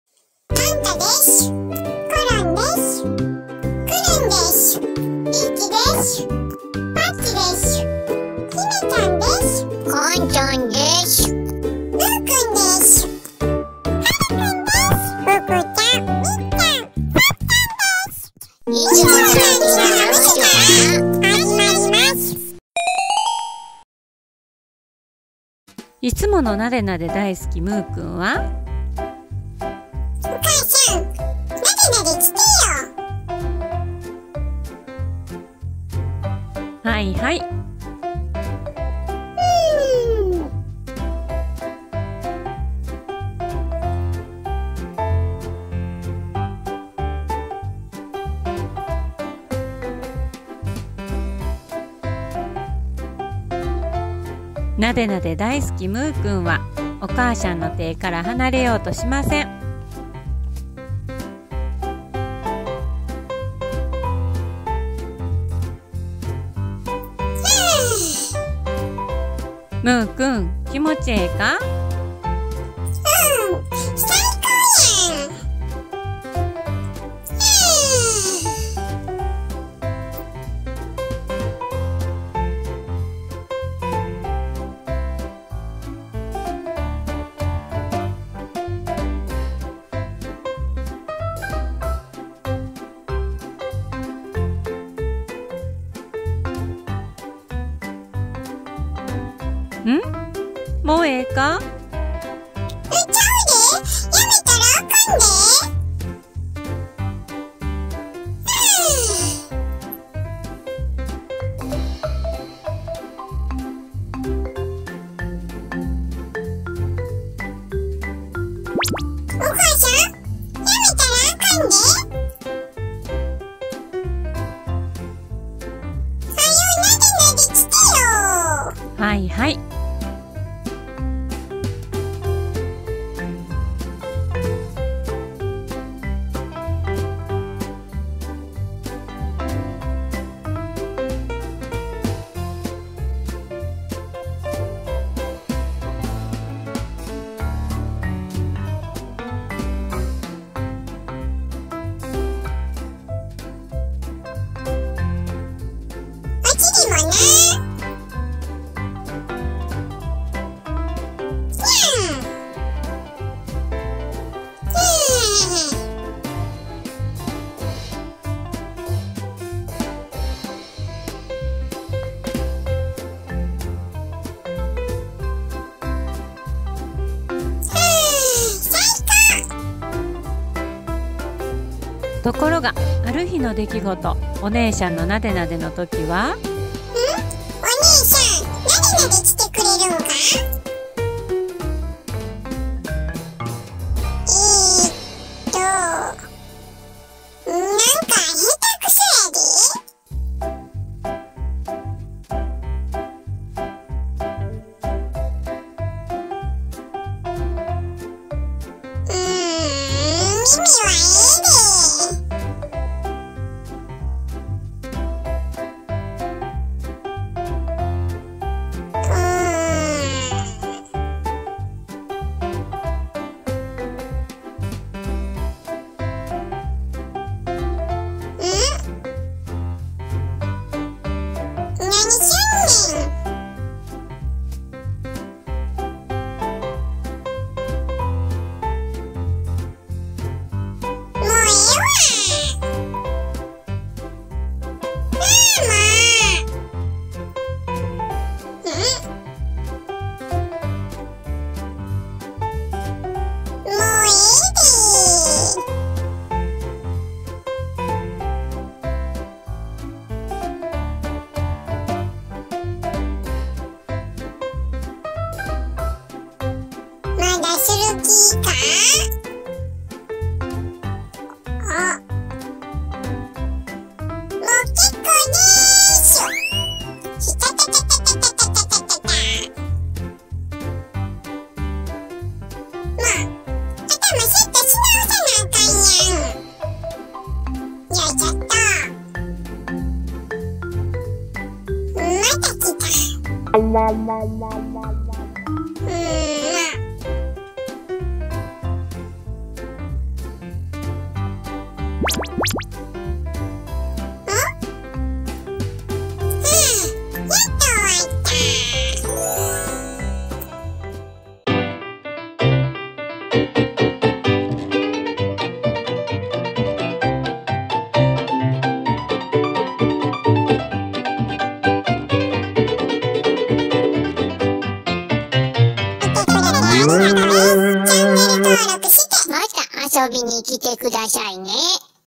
【ハムスター】なでなでは好きやけど～I like being stroked,but…～の巻
ハムちゃんとしては珍しく、なでなでが大好きなむーくんですが、おねえさんのなでなでは苦手😅💧
今日は喜んでなでなでされている時と、そうでない時の違いをみてみてくださいね🐹💦🐹💕